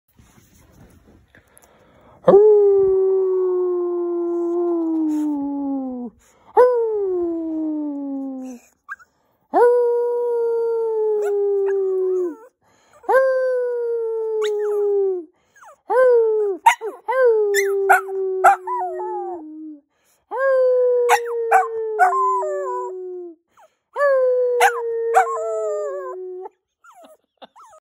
HOWL!!! Lightning the shorkie is a howler.
Lightning can howl when he misses his mom.